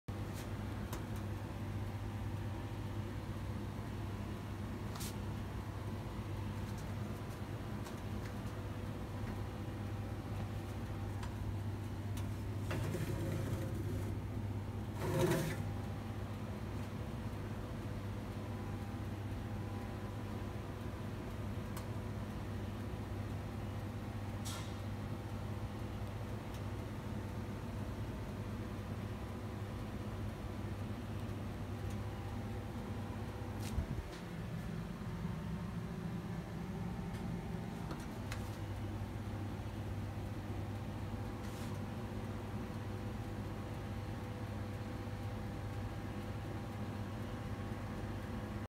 Cooked Sausage Live Cafe Shop 777+ ~ABDULL7H AL MA7UN | 777+ News | COVID-19 | Delizia Cafe | London
Cooked Sausage Live From Delizia Cafe Restaurant Kitchen High Power Oven  London 777+
Cafe shop